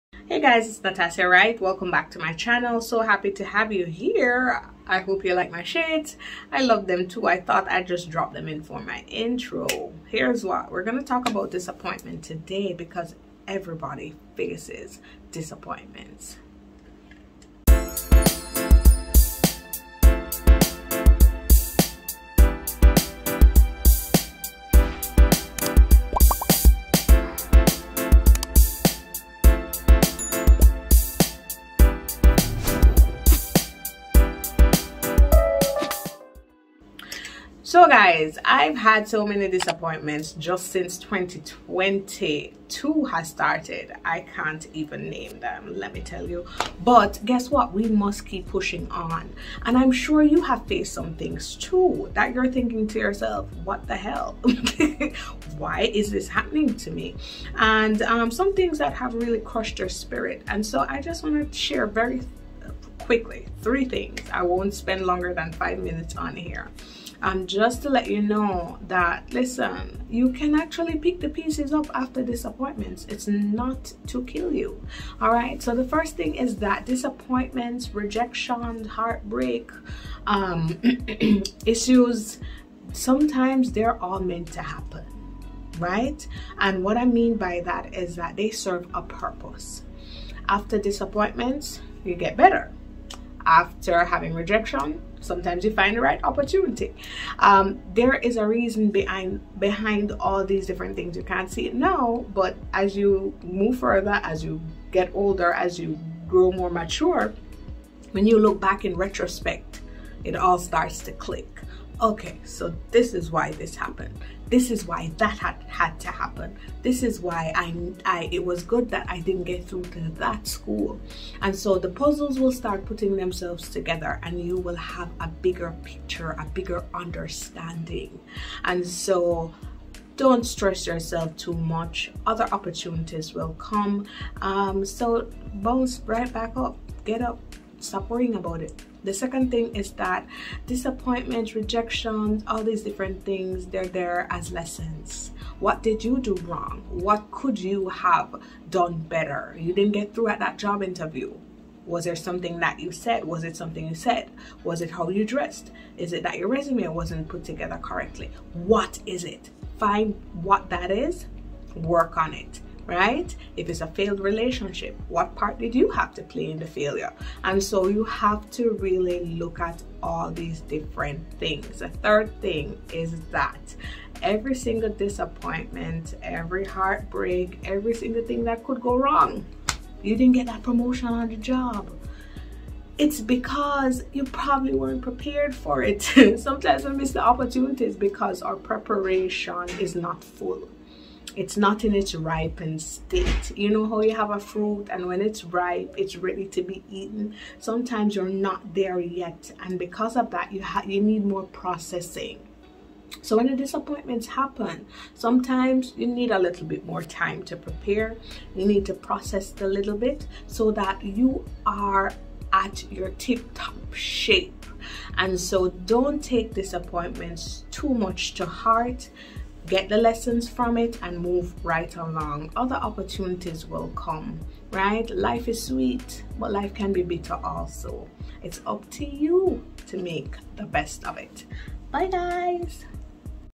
Dealing with Disappointments in #2022 ||| How to Handle Disappointments
Hey Guys!

Here's a short video of how I handle life's toughest challenges. Disappointments will come, but we need to respond effectively. Not by moping all day but by keeping it moving.

Here are the links for some of my other videos for you to check out!!!